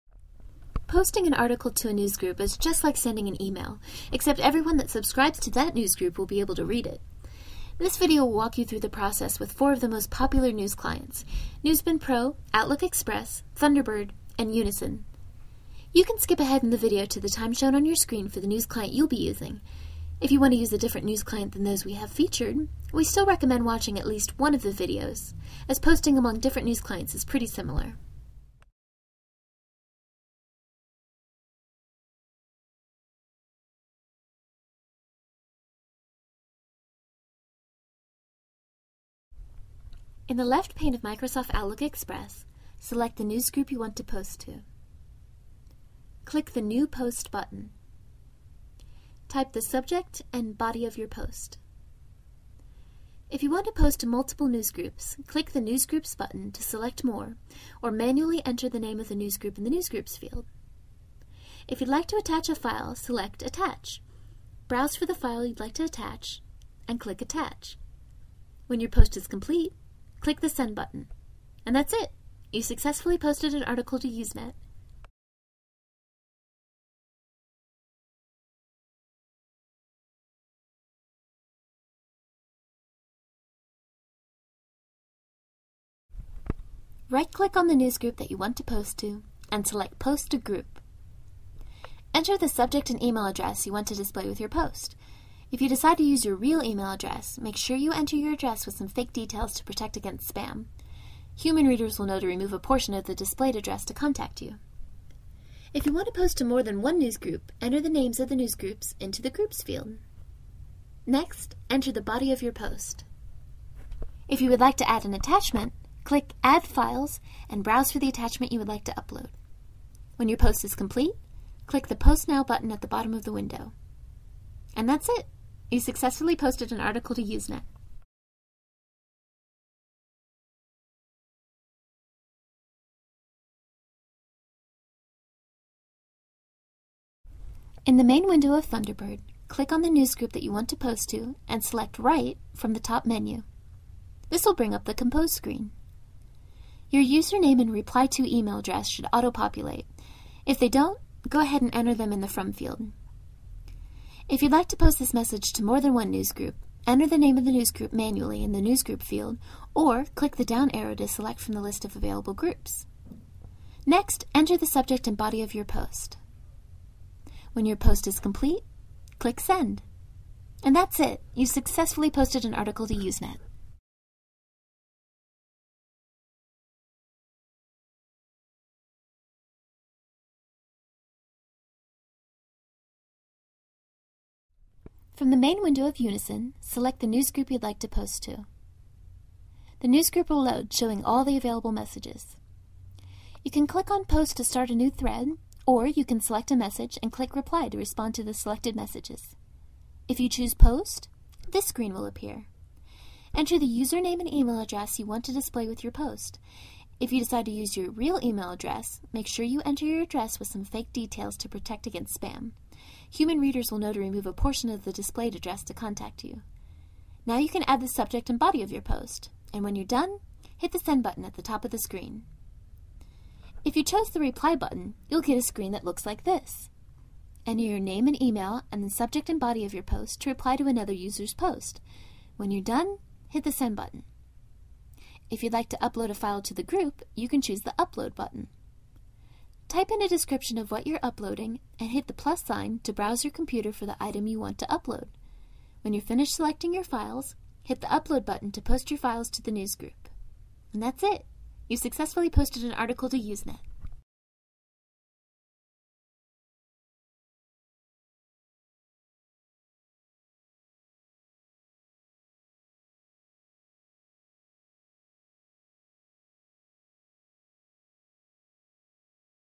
Posting to Newshosting Usenet using popular Usenet Apps
This video will walk us through how to post messages to Newshosting Usenet using other popular third party apps like, Thunderbird,Outlook Express and Newsbin Pro.